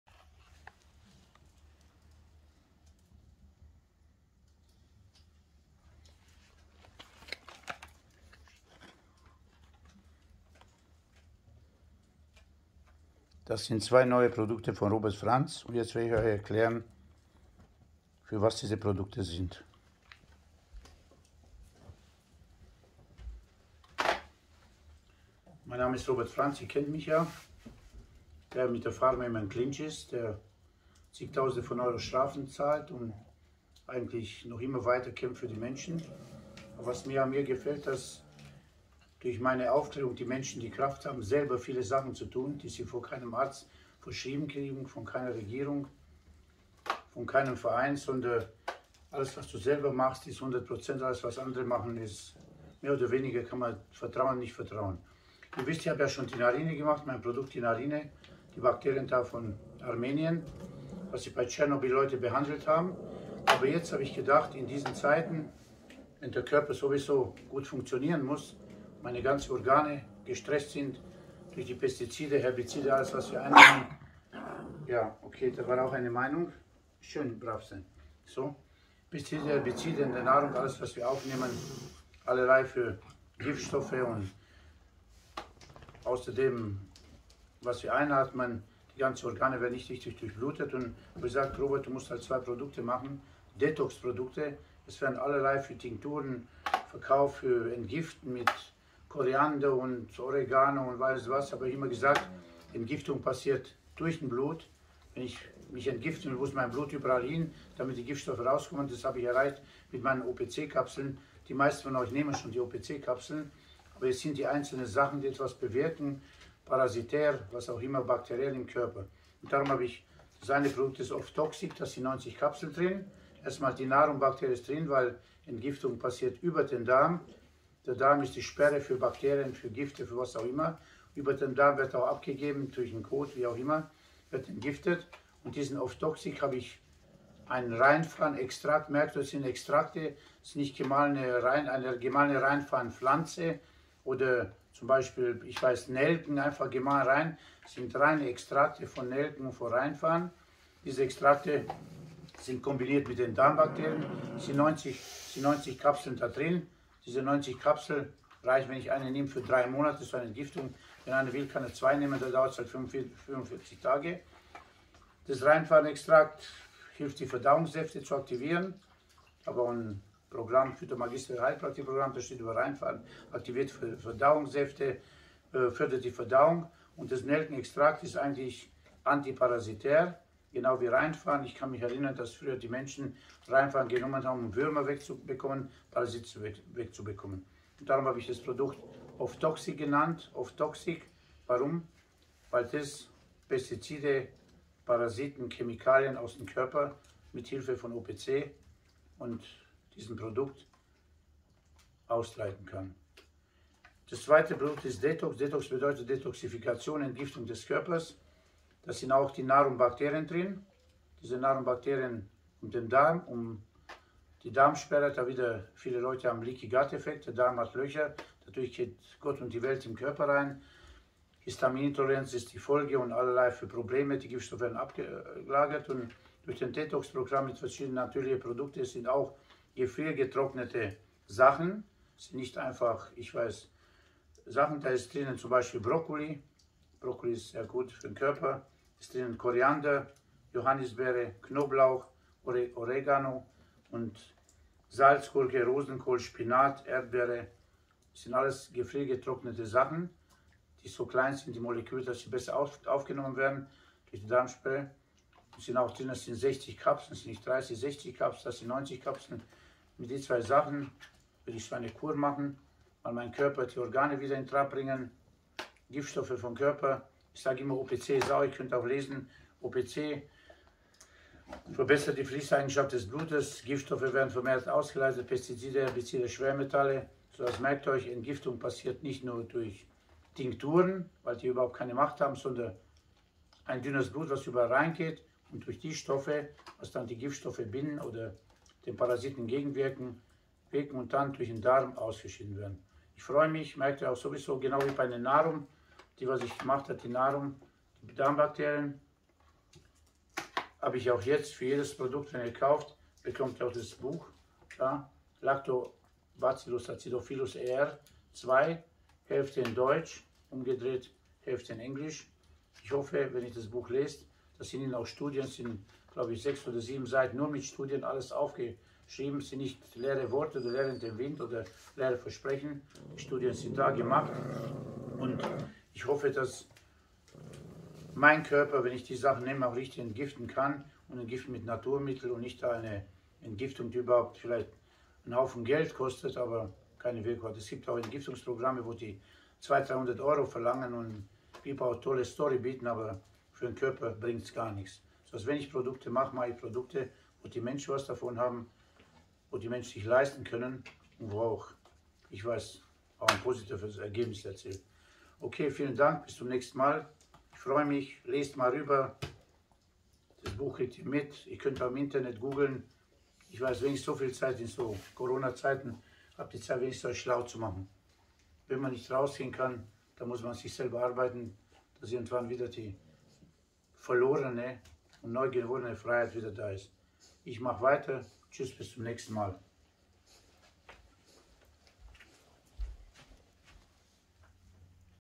Robert Franz: Detox und Off Toxic
Jede Entgiftung meines Körpers läuft auf natürlicher Basis aber immer mit der Einnahme von Opc und mit Hilfe meines Darms.